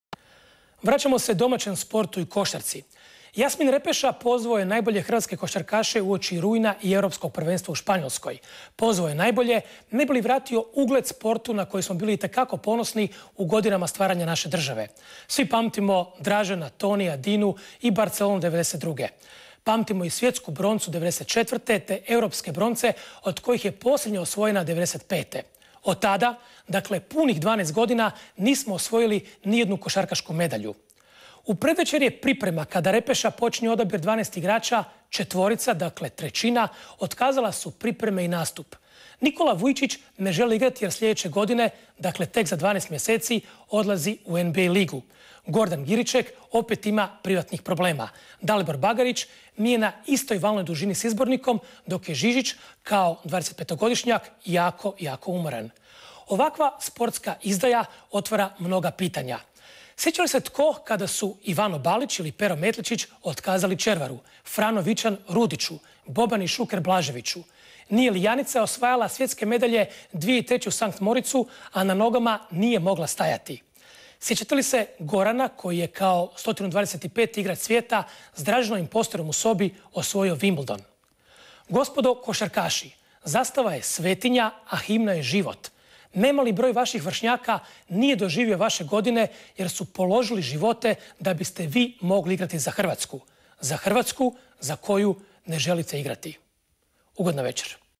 Bruno Kovacevic i kosarkasi
Bruno kovacevic odrzao lekciju kosarkasima